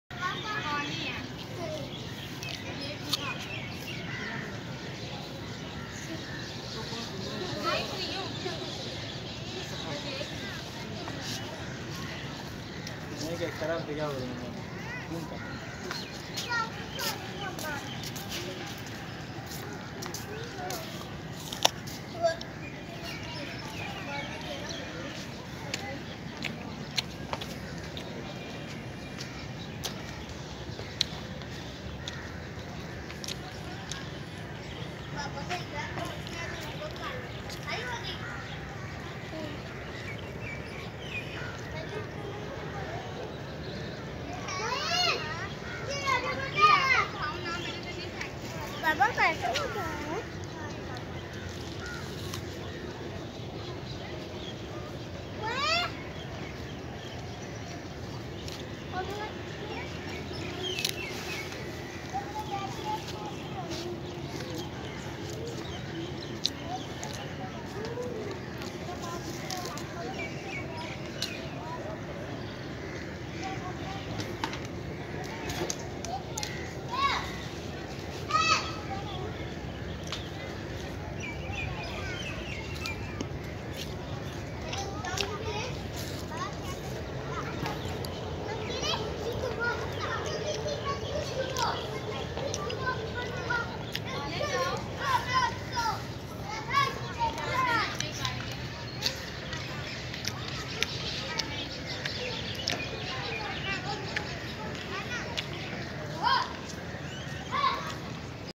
Beautiful Birds Views [ Shalamar Garden ]
The Shalimar Gardens are a Mughal garden complex located in Lahore, Pakistan. The gardens date from the period when the Mughal Empire was at its artistic and aesthetic zenith, and are now one of Pakistan's most popular tourist destinations